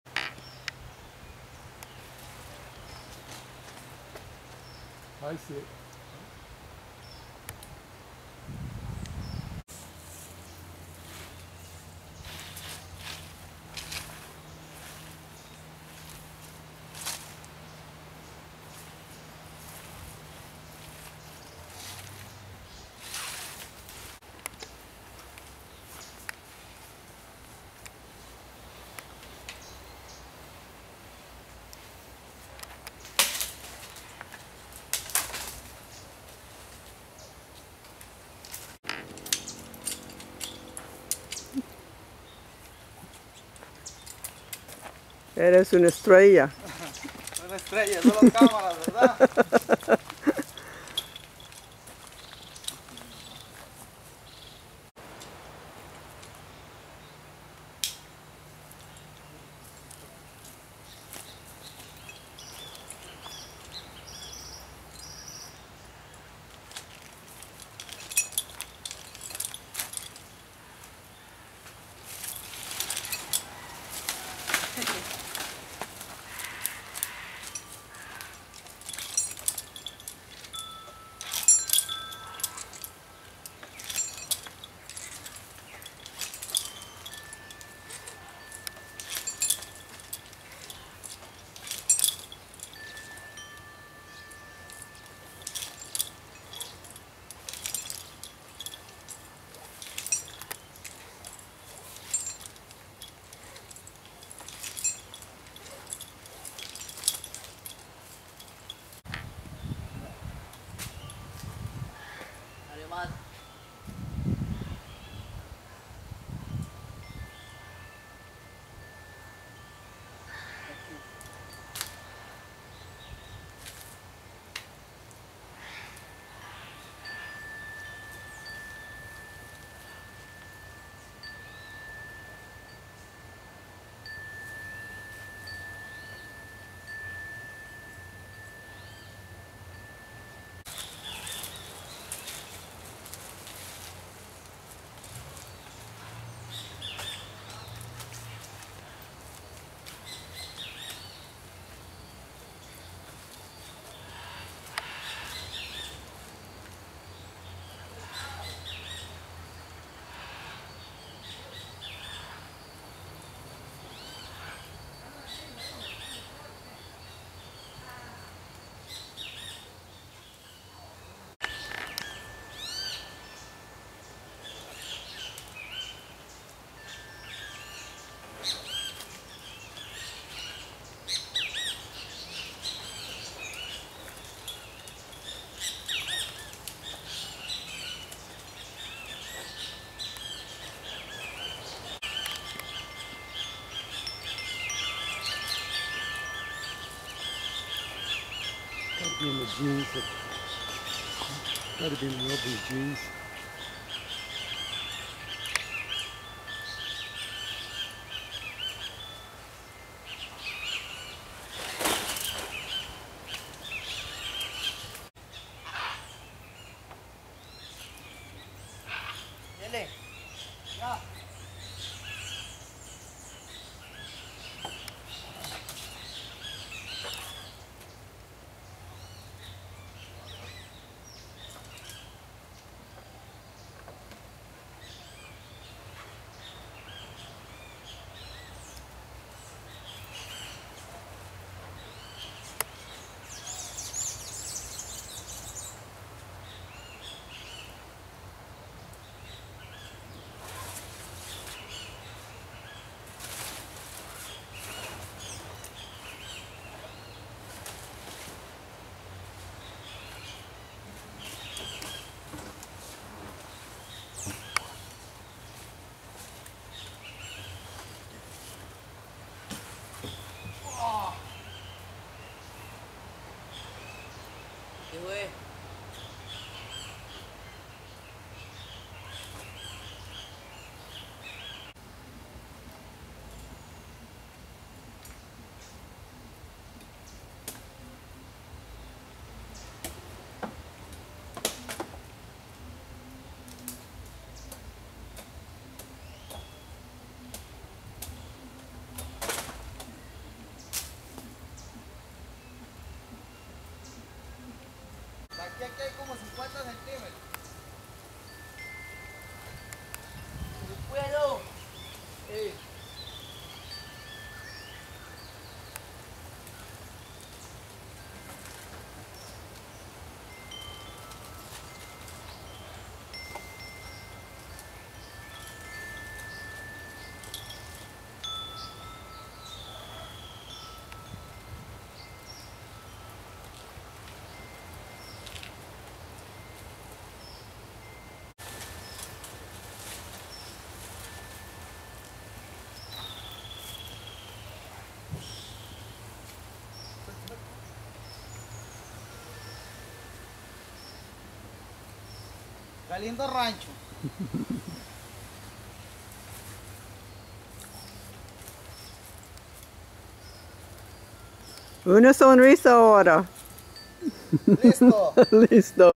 Macaw Nesting Box
Enrique and Fabian installing our Macaw nesting box at Casa Sammie, Tarcoles, Costa Rica